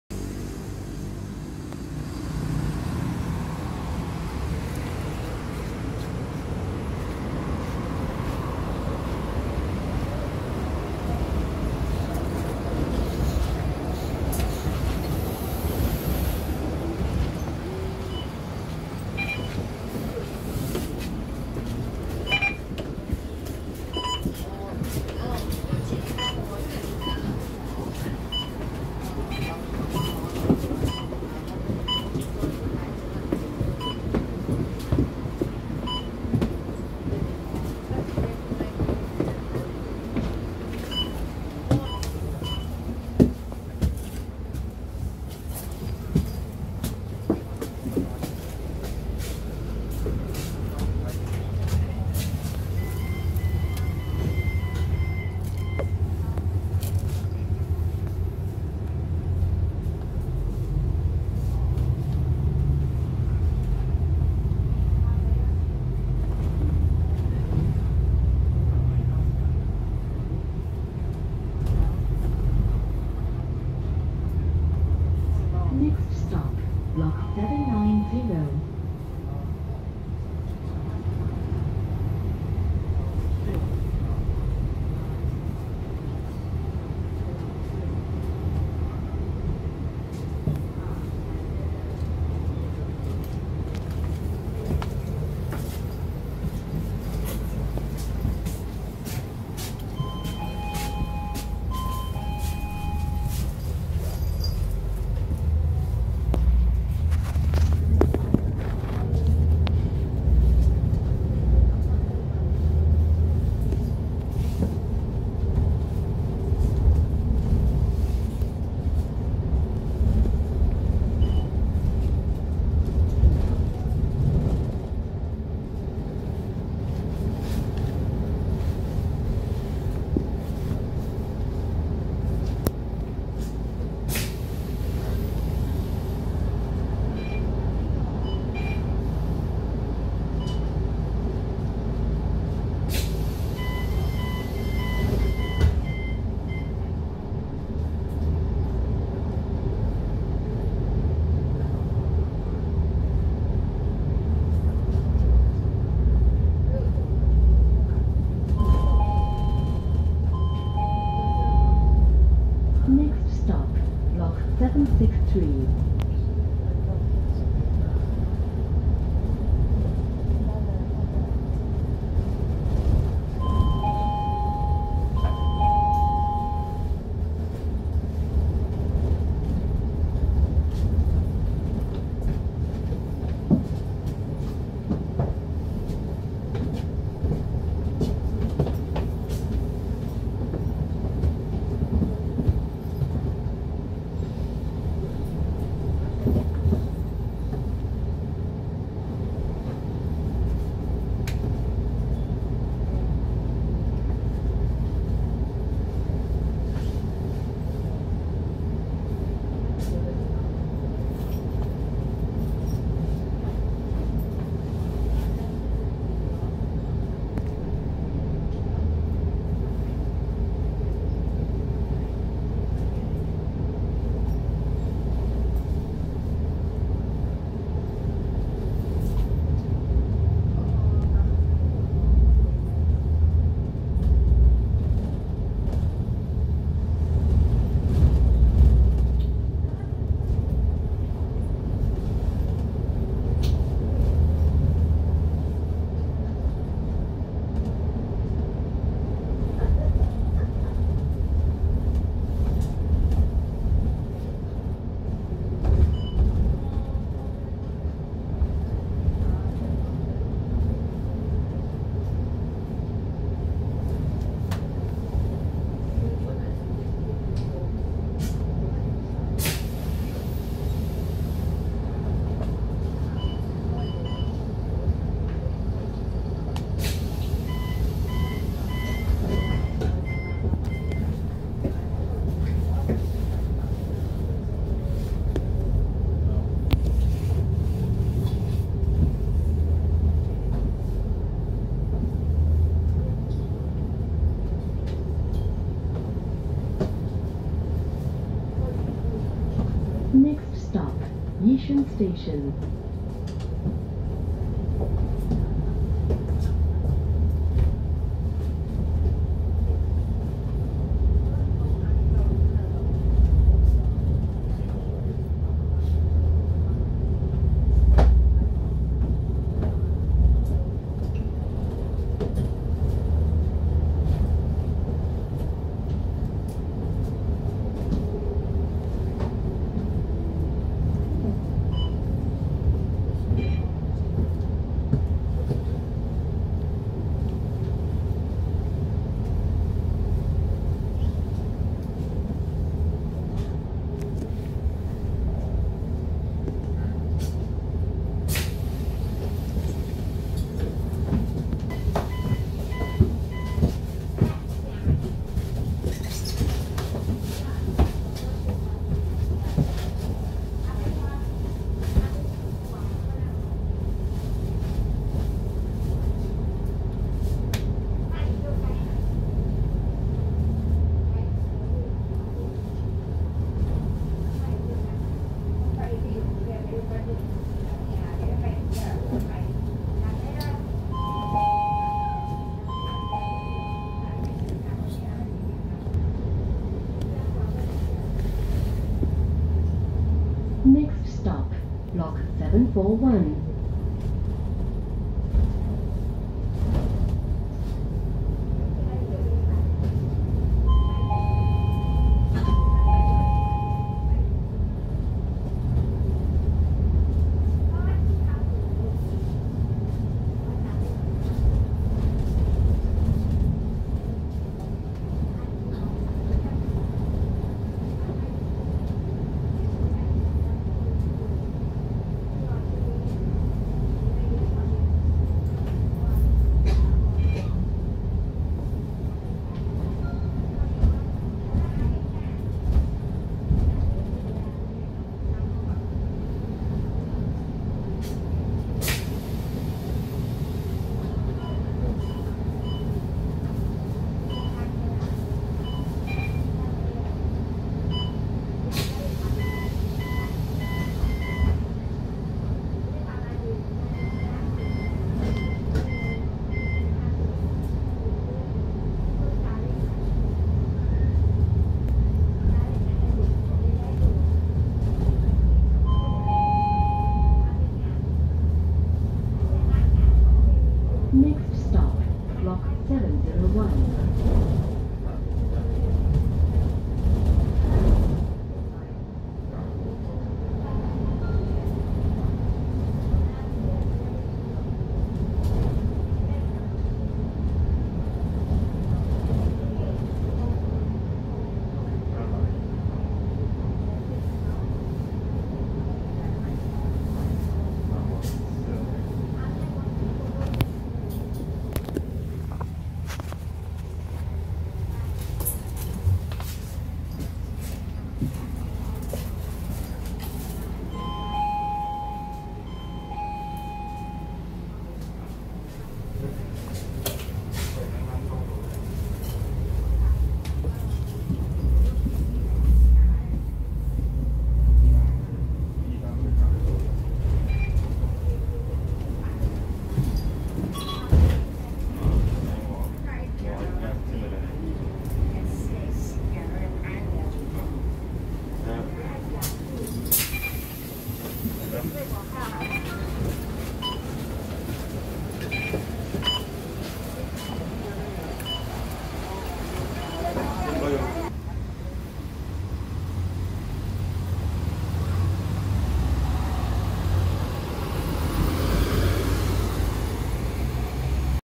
SG6155K Sv 969. MAN A95. Euro 6. (Batch 1) DD Bus. SG❤BUS.
- Bus Registration Plate: SG6155K

- Bus Contracting Model  (BCM) Under Land Transport Authority. (LTA).

- MAN ND323F A95 | Facelift. Euro 6 (Batch 1) Production Batch Double Deck Bus. MAN Lion's City DD. Gemilang Coachwork Bus Bodywork Design.

- Bus Operator: Tower Transit Singapore  (TTS).
- TTS Bus Depot: Mandai Bus Depot. (MDDEP).
- TTS Bus Service: MDDEP Service 969.
- TTS Current Bus Service: MDDEP Service Flexi-Deployments.
- LTA BCM Bus Package : Sembawang - Yiishun. (Mandai Bus Package). Tower Transit Singapore. (TTS).

- Location: Yishun Ave 2 - Bef Khatib MRT Station. (Bus Stop).

- Destination: Yishun Ave 5 - Blk 701. (Bus Stop).

- Tower Transit Singapore. (TTS). Bus Service 969 (Direction 2): Tampines Ctrl 1 - Tampines Bus Interchange Towards Woodlands Square - Woodlands Regional Integrated Transport Hub. (WITH). Bus Interchange.

- Tower Transit Singapore. (TTS). Bus In (LTA) Lush Green Bus Livery. SG❤BUS. Moving Ahead Together. Without Bus Advertisement.

- Bus Journey Rating: 95% / 100%.

- Date & Time Filmed: Tuesday. 26 October 2021. @ 22:46 PM.